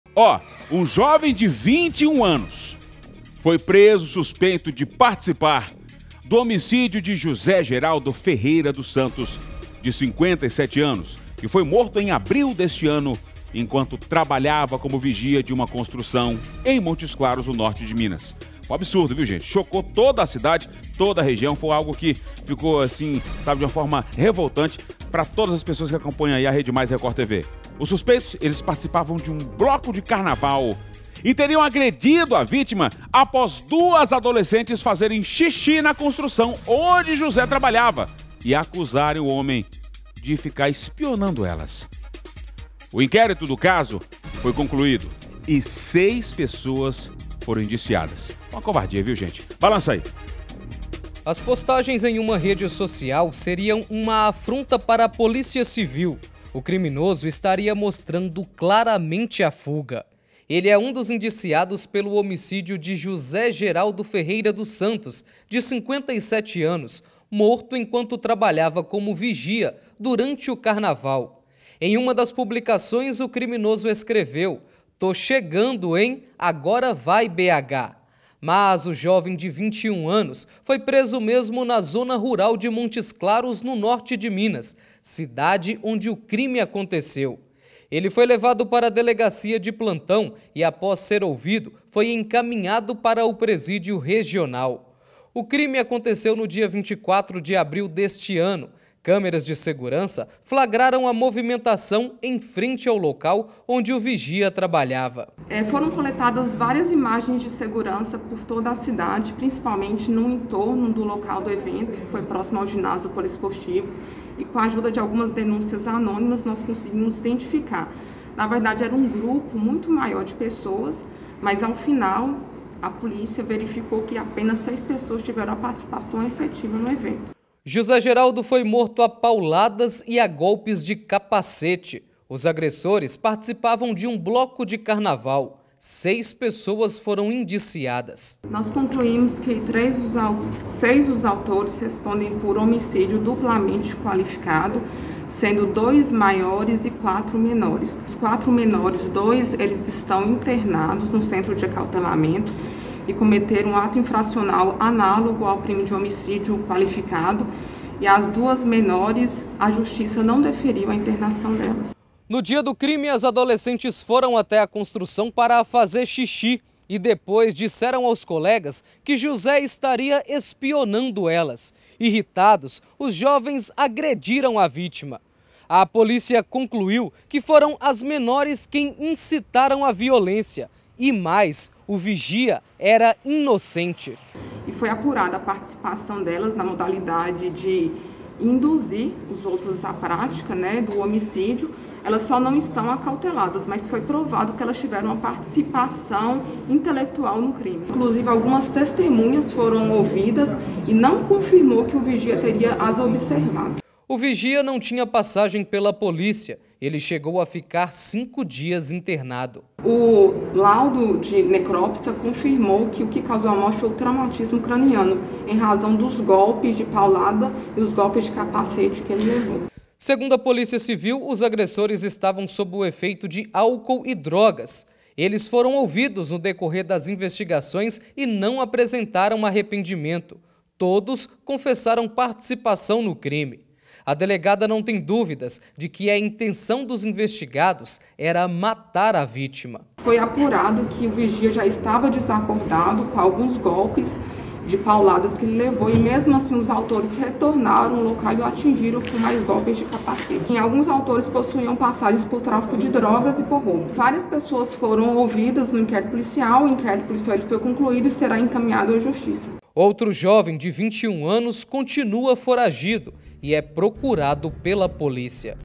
Jovem é preso suspeito de matar vigia em Montes Claros
Crime aconteceu durante Carnaval deste ano